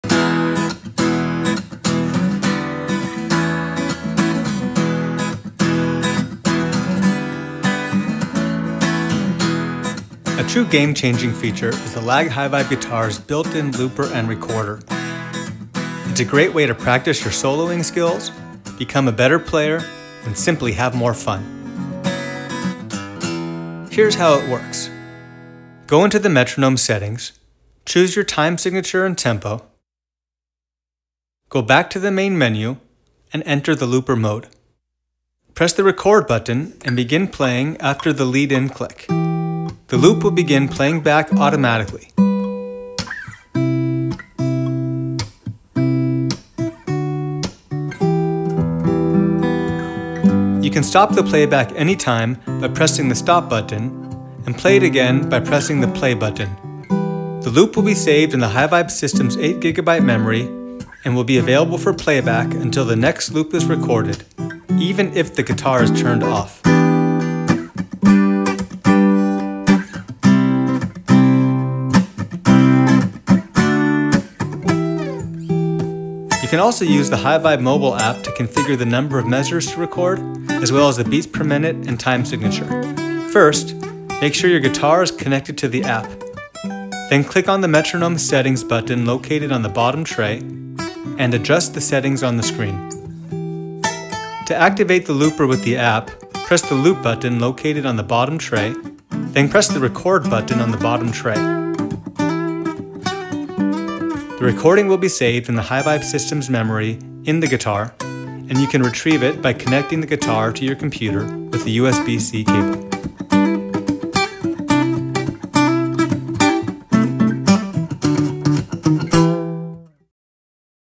The Lâg HyVibe Smart Guitar: built-in looper and recorder
Use our built-in looper for recording your own backing tracks to jam over and use the HyVibe Mobile App to record your playing. Share your recordings with our USB C data port. The Lâg HyVibe Smart Guitar will take your creativity to new levels.

Remember, there are no speakers in this video, all the sounds come from the body of the guitar!